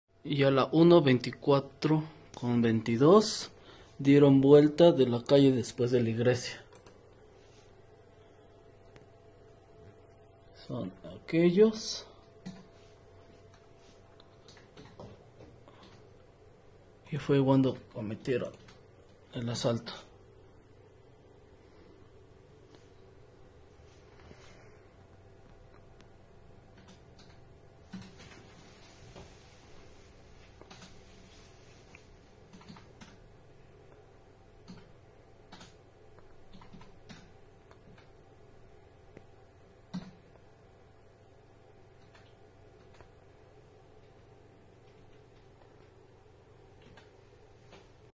Vecinos del Ojo de Agua en Texmelucan denuncian otro asalto a ciudadano
Fue después de las dos de la tarde de este miércoles que vecinos de la Colonia Ojo de Agua de San Martín Texmelucan denunciaron a través de redes sociales el atraco a un ciudadano que se encontraba dentro de su vehículo en la vía pública.